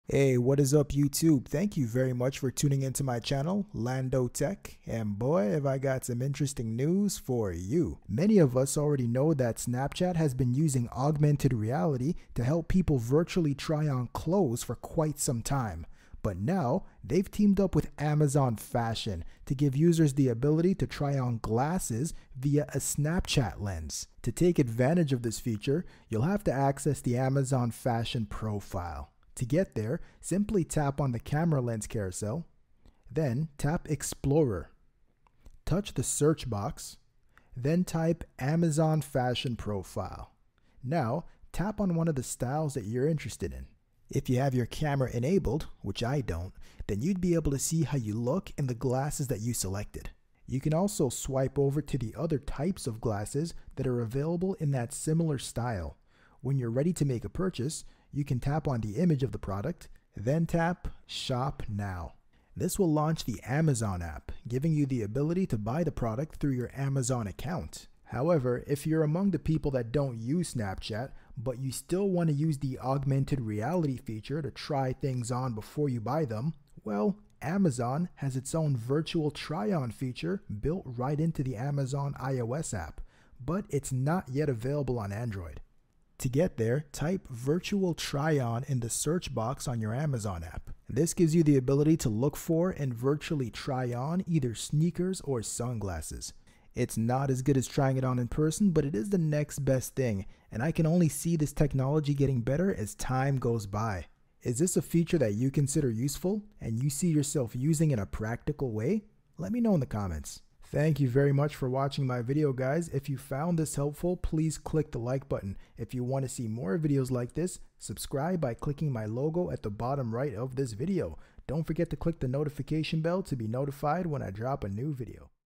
How to Buy Sunglasses on Snapchat via Amazon Fashion Virtual Try on
In this tutorial I show you how to virtually try on, using augmented reality, then purchase sunglasses and shoes on Snapchat via Amazon.